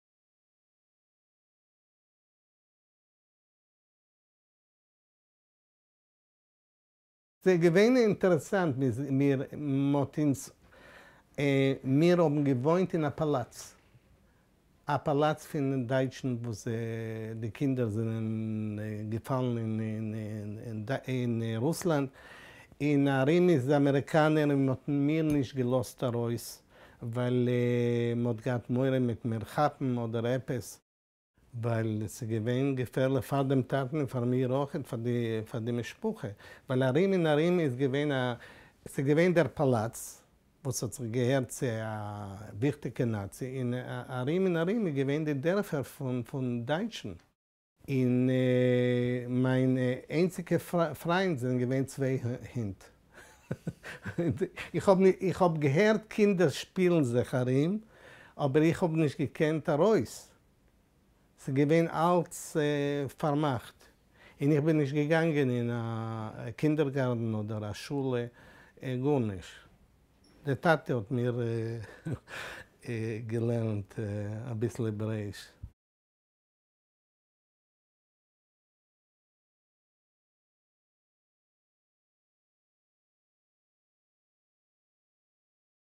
Growing Up in Berlin After World War II
Zvi Mann, son of Yiddish writer Mendl Mann, describes what his life was like as a child in Berlin immediately after World War II.